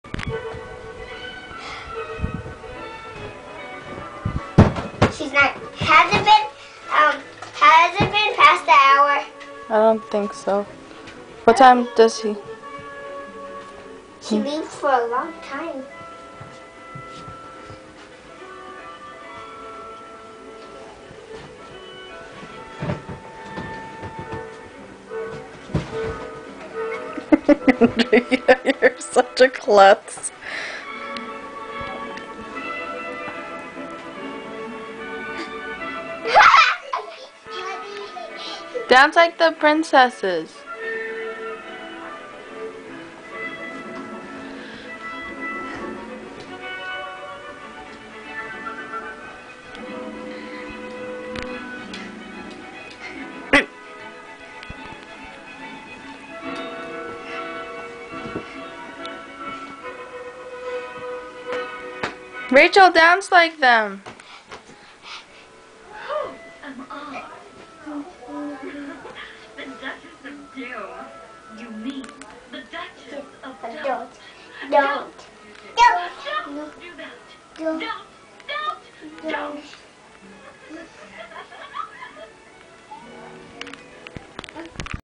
3 dancing fair princesses
the girls were dancing with the movie. ahahaha